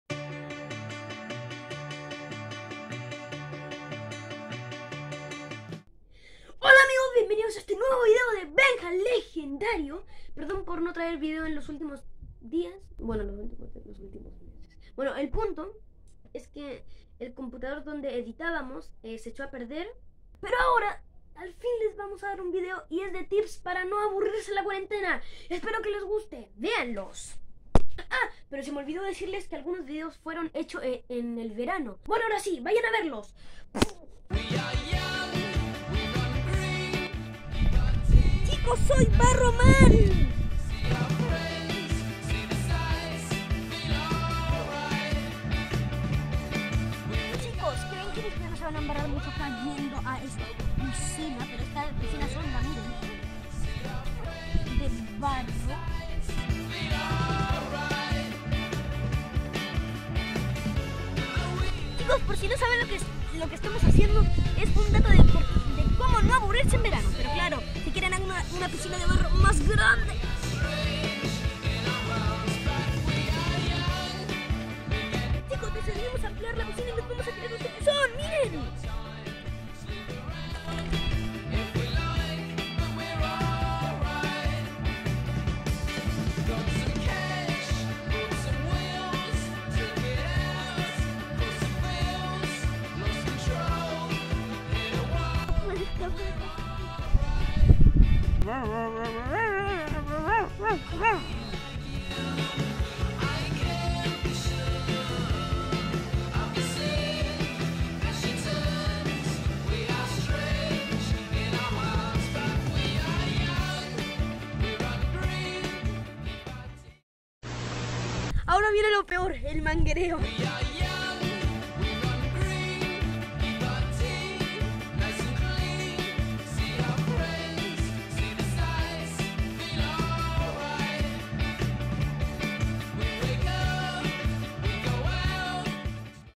Tips para pasar la cuarentena N°1: Baño de barro
Hola amigos de BenjaLegendario! Por fin estamos de regreso!
Durante estas semanas les daré consejos para pasar la cuarentena y Divertirse, como en este caso es un baño de lodo…Este video lo grabé en el verano, pero igual aplica para los días con sol.
Soy benja Legendario y pronto regresaré con Lipi!